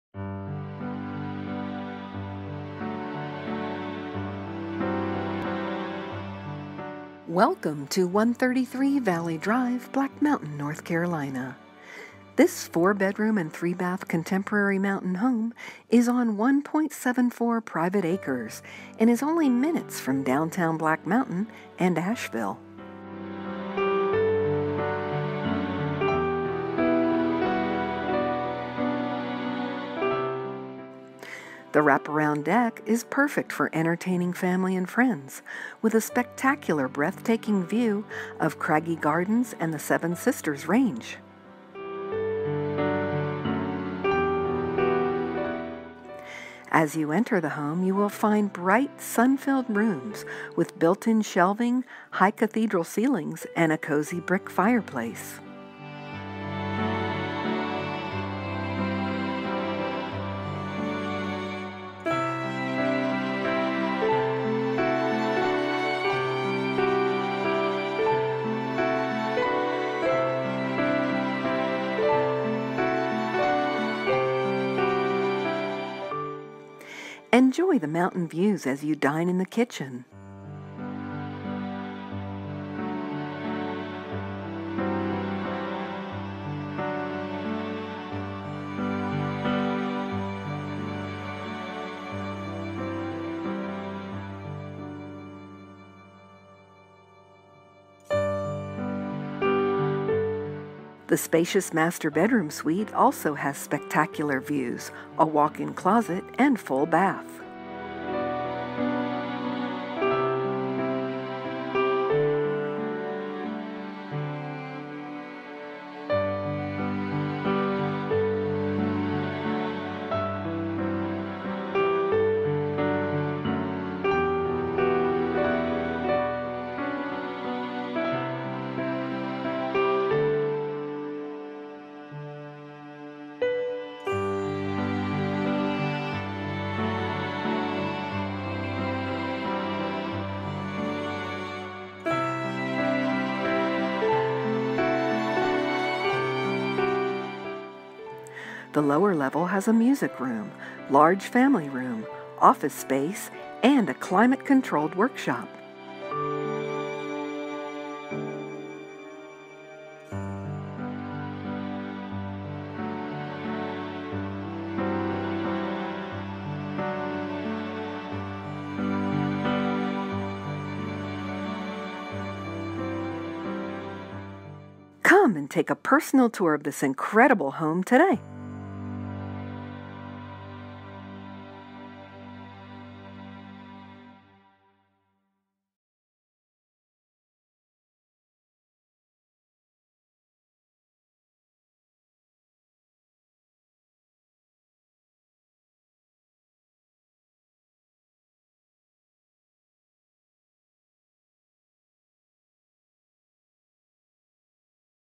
133 Valley Drive, Black Mountain, NC
Take a high definition video tour of this beautiful property for sale:
133 Valley Drive, Black Mountain, NC